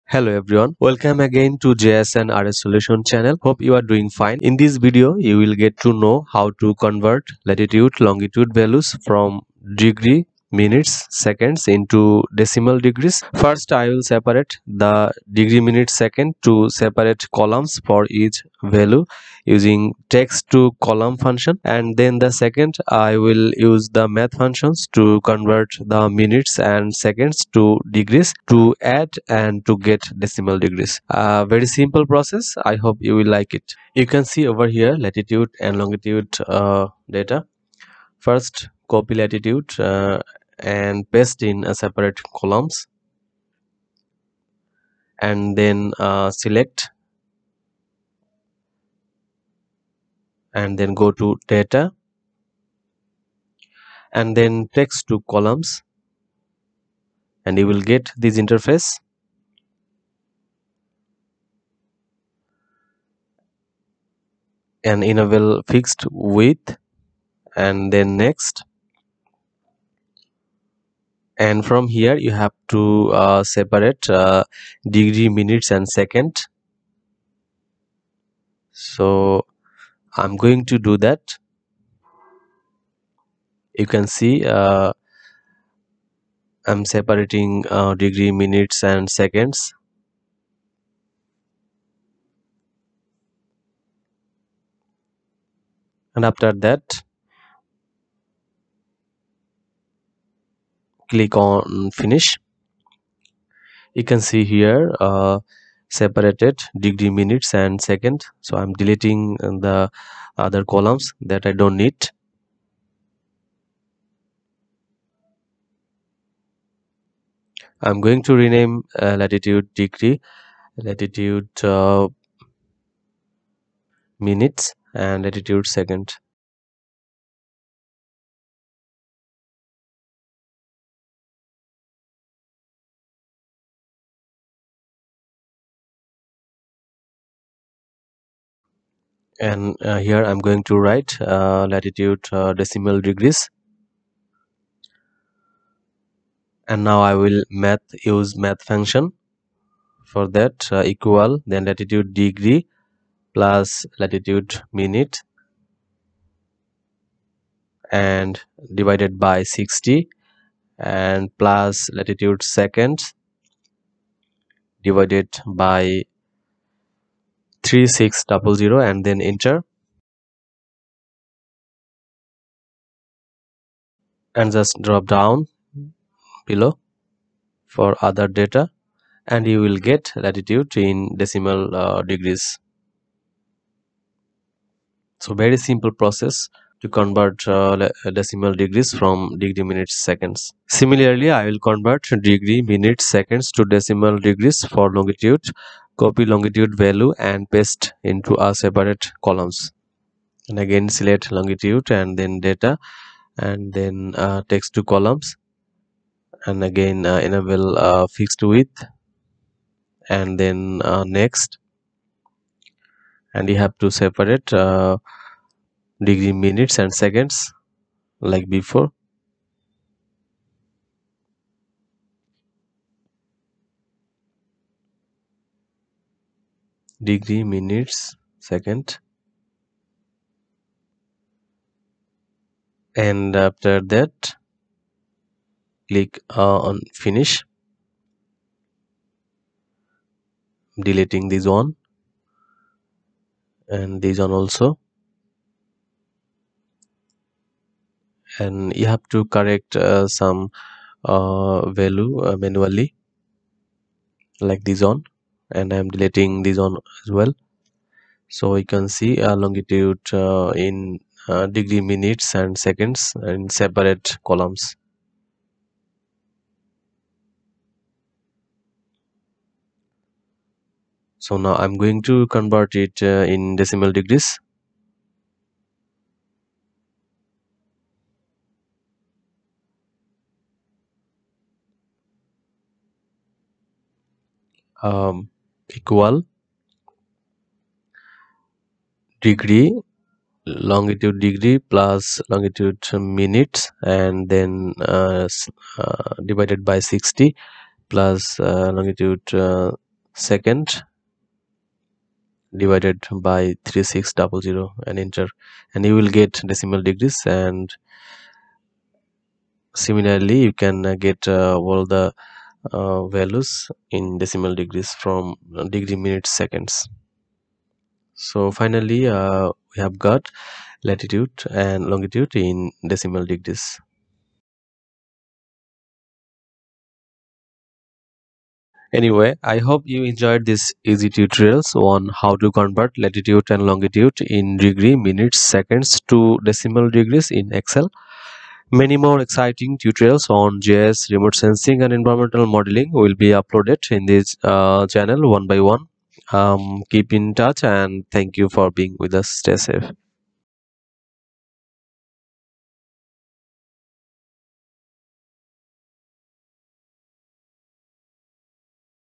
How to Convert Lat Long in Degree, Minutes, Seconds (DMS) to Decimal Degrees (DD) in Excel
Hello everyone, Welcome again to GIS and RS Solution channel. Hope you are doing fine.
In this video you will get to know how to convert Latitude Longitude values from Degree, Minutes, Seconds into Decimal Degrees.  First I will separate the DMS to separate columns for each value using text to column function, then the second I will use the math functions to convert the minutes and second to degrees to add to get decimal degrees. Very simple process. I hope you will like it.